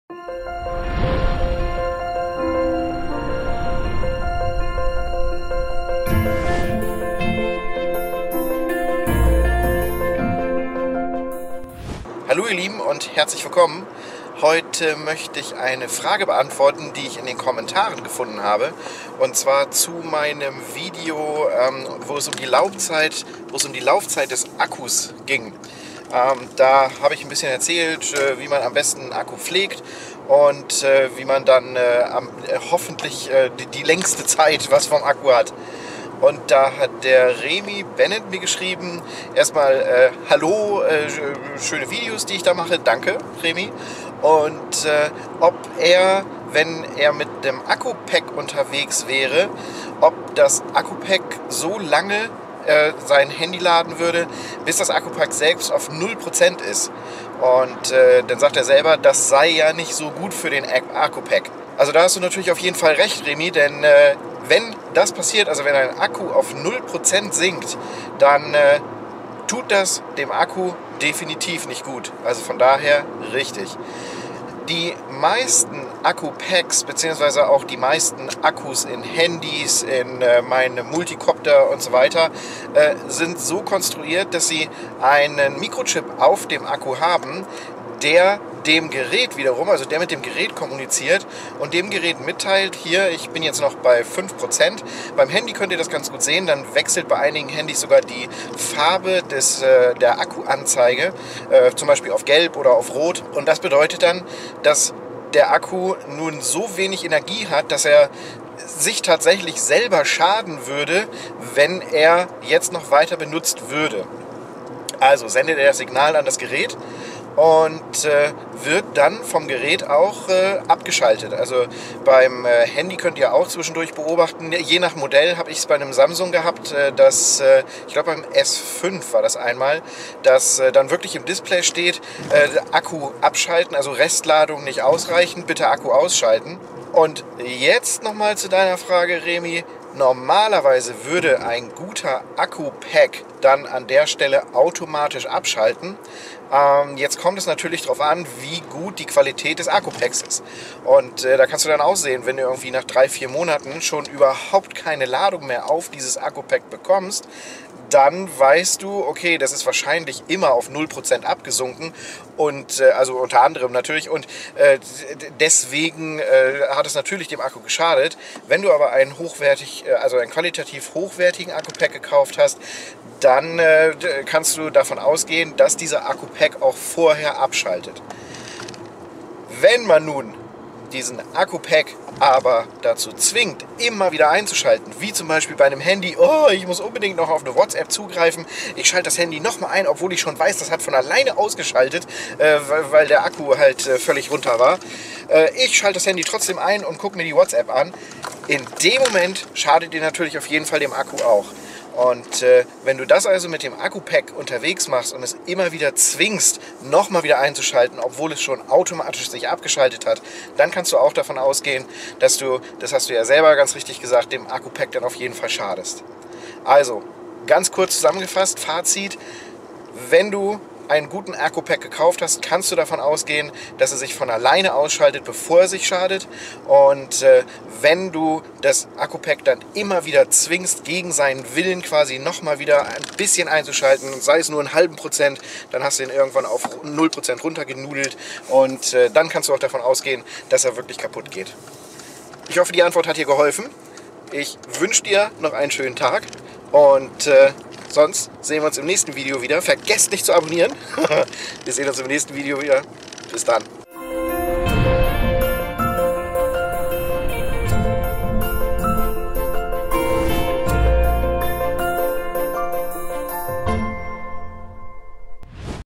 ❓Kann ein Akku-Pack auf 0% Ladung fallen und ist das schädlich? | Eure FAQs
✔ Von Zeit zu Zeit beantworte ich themenbezogene Fragen meiner Follower aus den Kommentaren.
Remi Benet: "Wenn man mit ein Accu Pack unterwegs auflädt, kann sich das Accu Pack bis 0% entleeren? Das wäre ja dann nicht so gut für die Lebensdauer des Accu Pack denke ich?"

✔ Dieses Video enthält KEIN (bezahltes) Product Placement!

☂ Links können Partnerlinks sein!

Ich habe dieses Video aus eigenem Interesse gedreht. Ich bekomme kein Geld von Amazon!